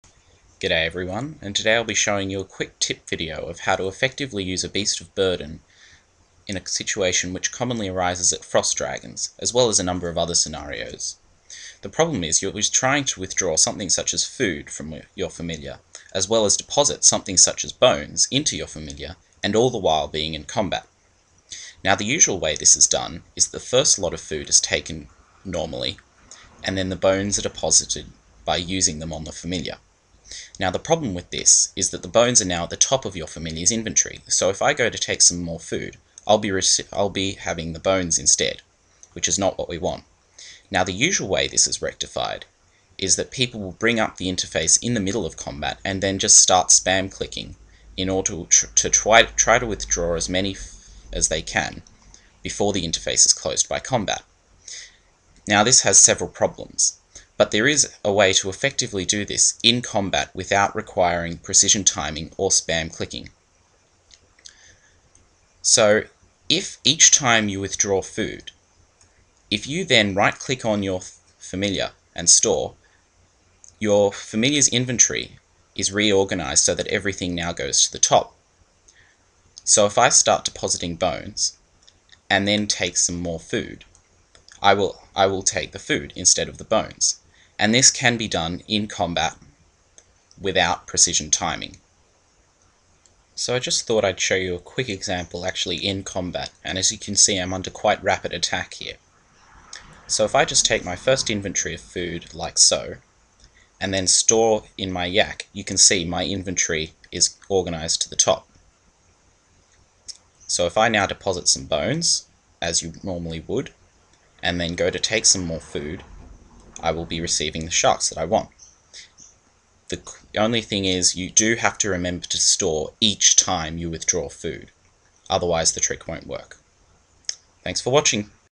Effective BoB use in combat
A quick tip when using a beast of burden in combat. This will work for any beast of burden, not just a pack yak, and will also work regardless of how much you are being attacked.